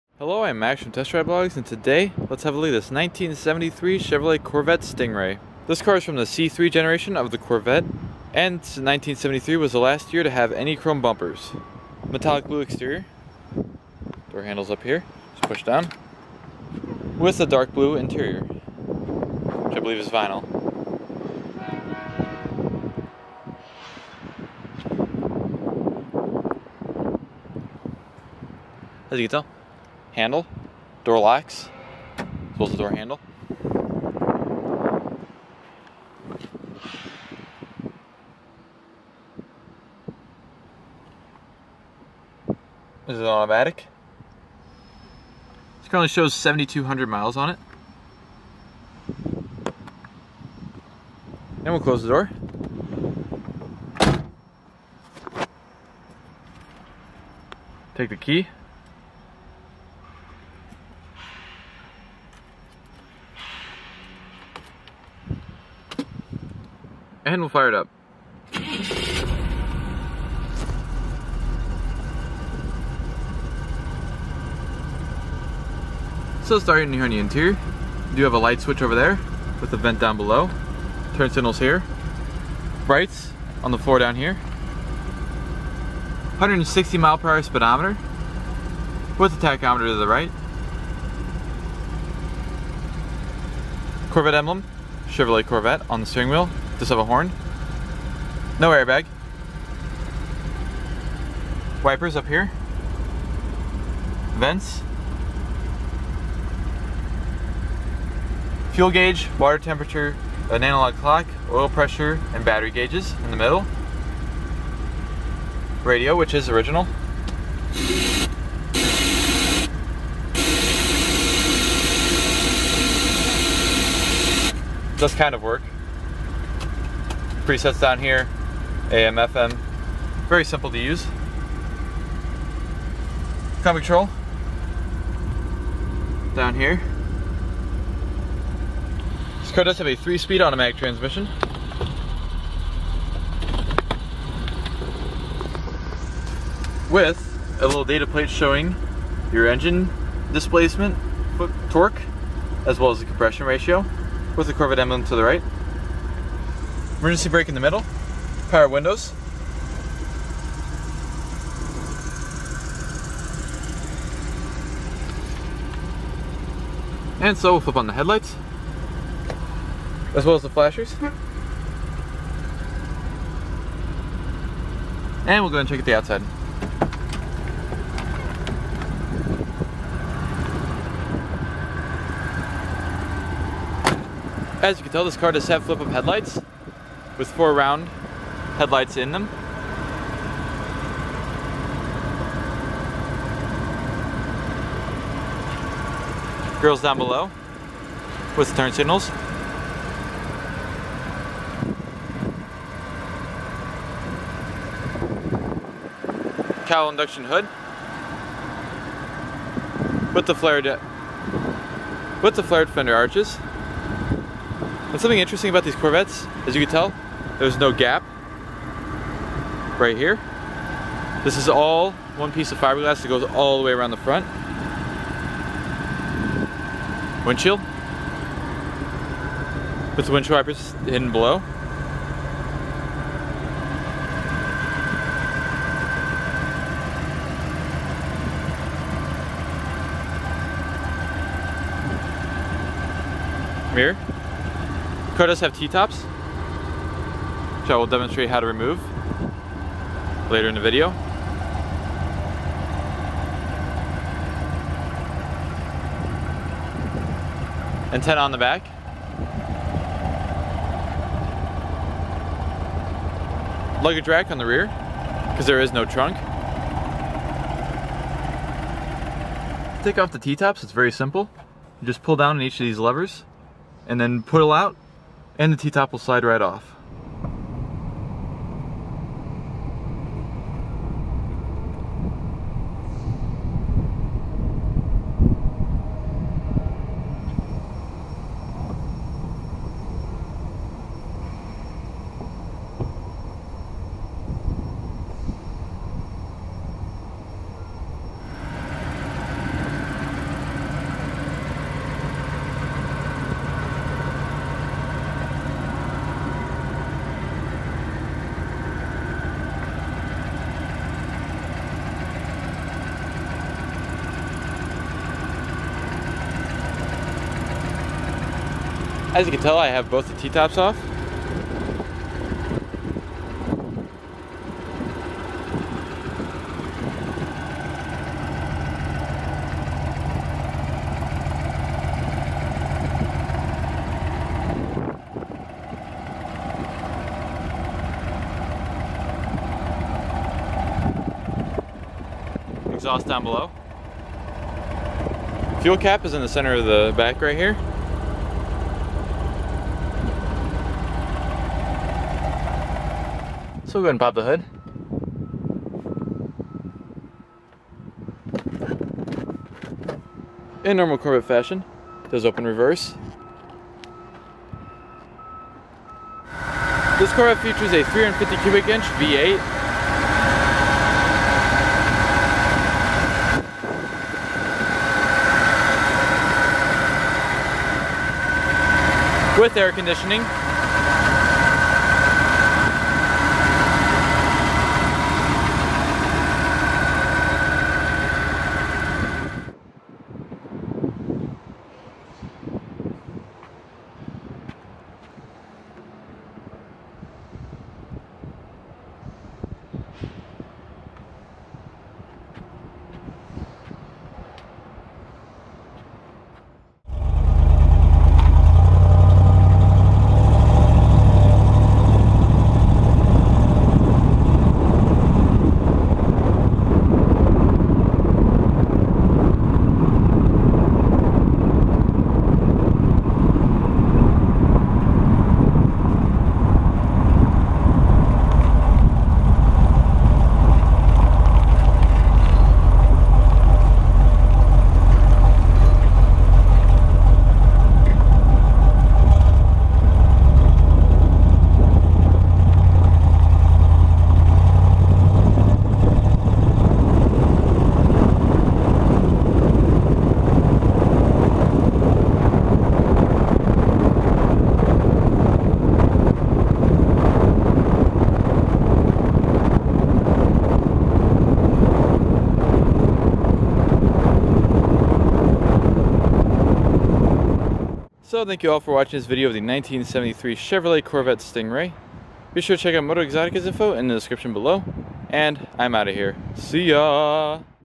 1973 Chevrolet Corvette Stingray Coupe | Full Tour, Start Up, and Test Drive
Vehicle provided by MotoeXotica.

tdbyt